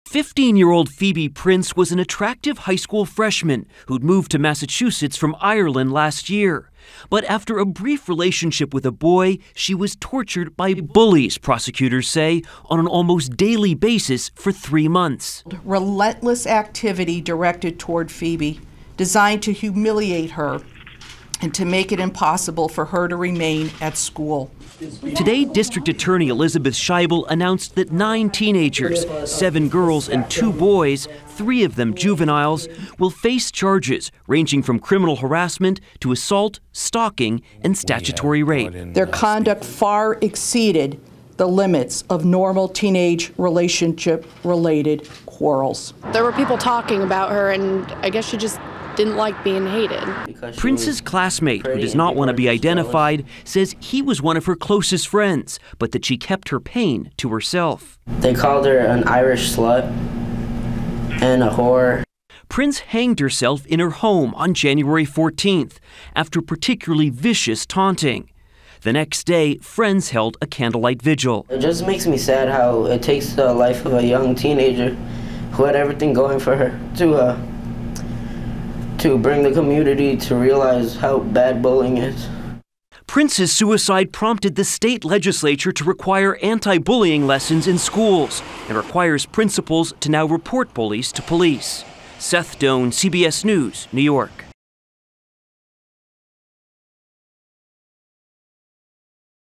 Bullied to Death
Officials in Mass. have charged nine teens over the alleged bullying and harassment of 15-year-old Phoebe Prince, who committed suicide after constant torment.  Seth Doane reports.